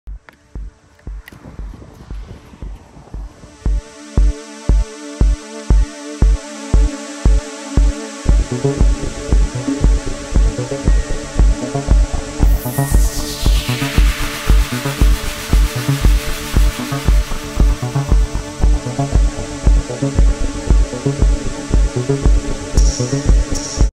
Langrend på Vejle golfbane
Sne, sol og langrend på Vejle Golfbane. What’s not to like ❄️🌞⛷